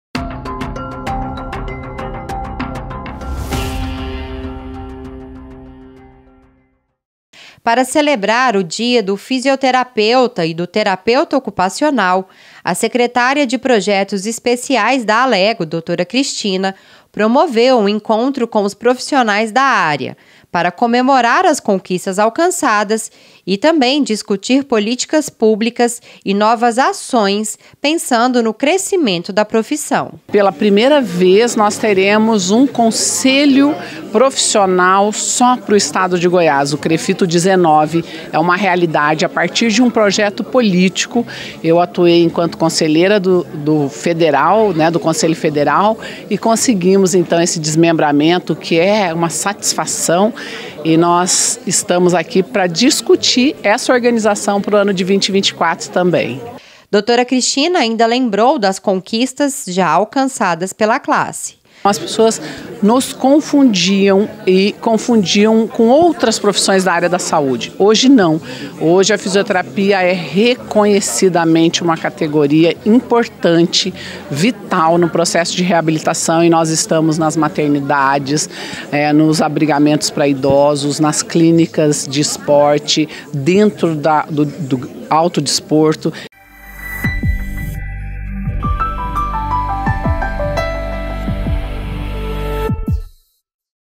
Alego News: Dia do Fisioterapeuta e do Terapeuta Ocupacional (27/10/2023)
Para celebrar o dia do fisioterapeuta e do terapeuta ocupacional, a secretária de projetos especiais da Alego, Dra.Cristina, promoveu um encontro com os profissionais da área, para comemorar as conquistas alcançadas, e para discutir políticas públicas e novas ações, pensando no crescimento da profissão.
Dra.Cristina lembrou das conquistas já alcançadas pela classe.

Confira mais na reportagem de Monalisa Carneiro.

outras sessões, reportagens e entrevistas na programação da TV ALEGO - canal digital 3.2 e canal 8 da NET Claro TV (em Goiânia).